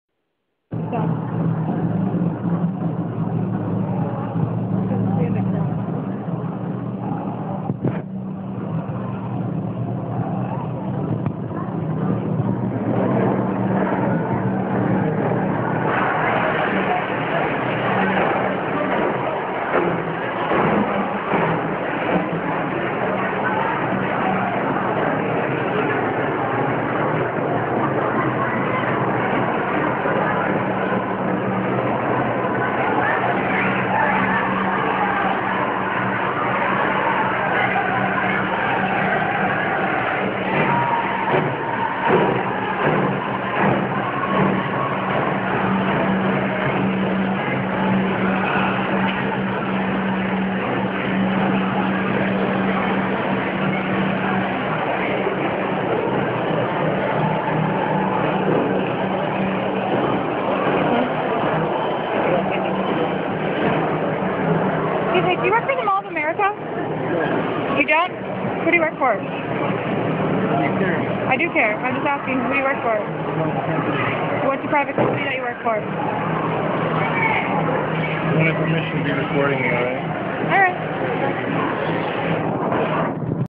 Idle No More: Mall of America creeper corporate videographer 12/30/2012 [2/2]
Weird private org videographer with an earpiece denies working for Mall of America. He seemed focused on longtime AIM (American Indian Movement) leader Clyde Bellecourt. Video contributed by a friend (Creative Commons).

At the Mall of America, Indigenous communities and their supporters gather for a flash mob in support of Idle No More protests and direct actions against PM Stephen Harper. Many natives were in town for the new years PowWow at the Minneapolis American Indian Center. Video shot by a friend.